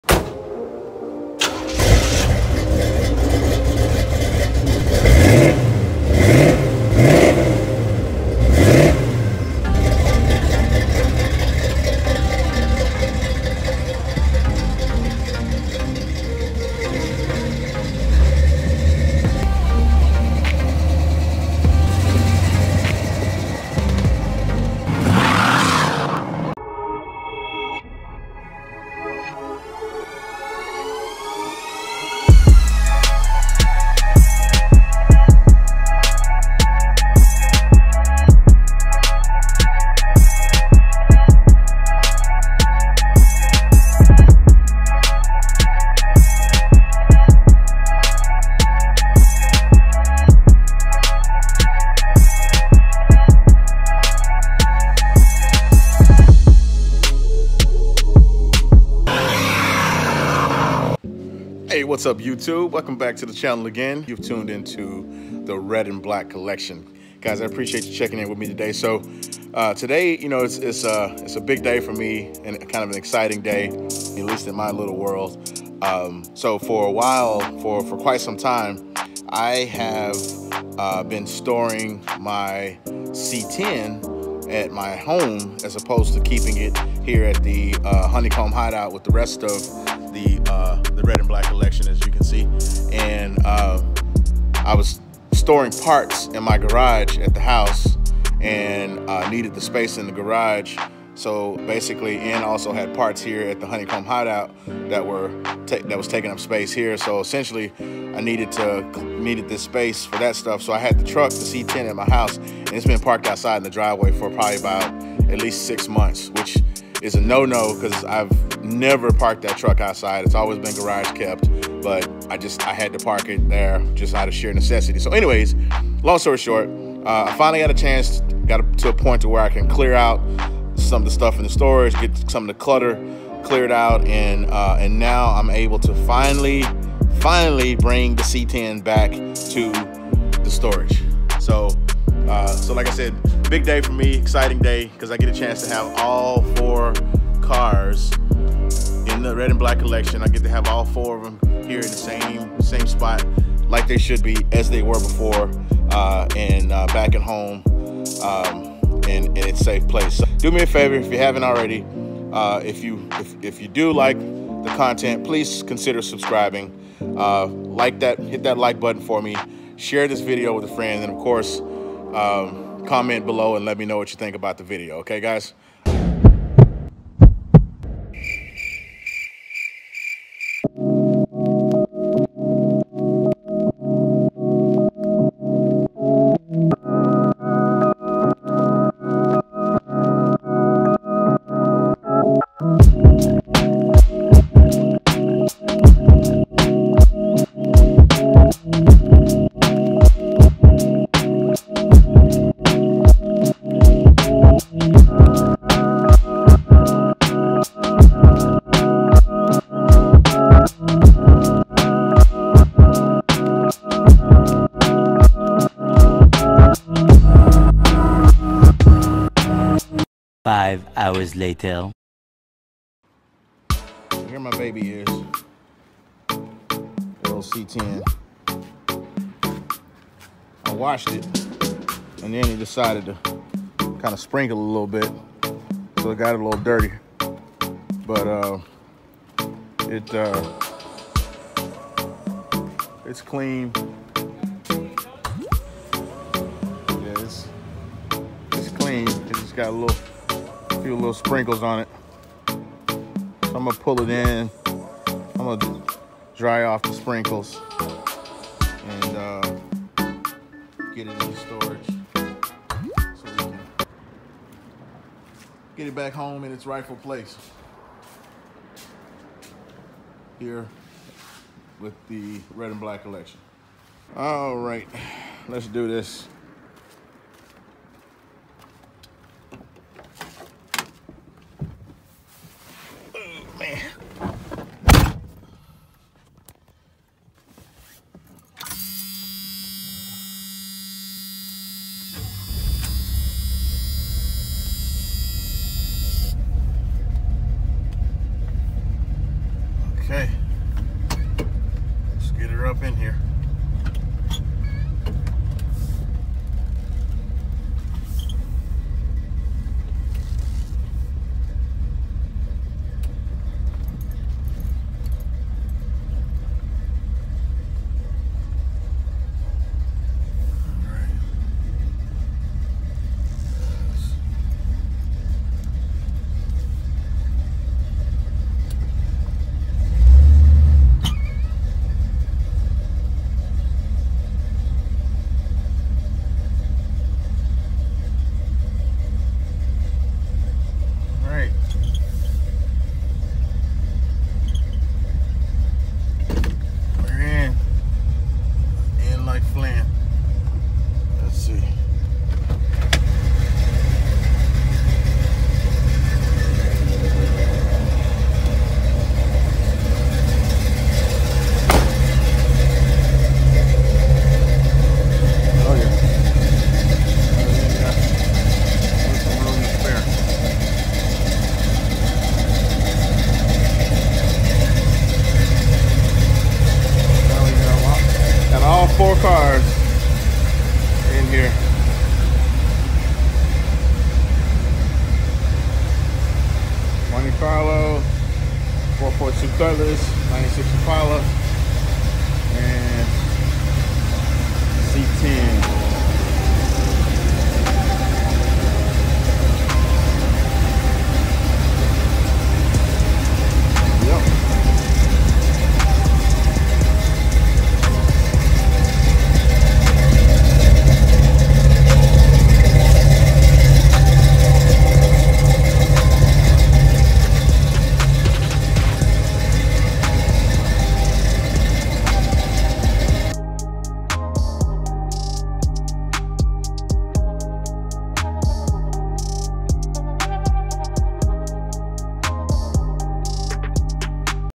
The Black Box is back! The C10 is reunited with the Red & Black Collection at the Honey Comb Hideout
Big day for the Red & Black Collection. The C10 is finally reunited with the rest of the collection at the honey comb hideout. The Black Box is finally home!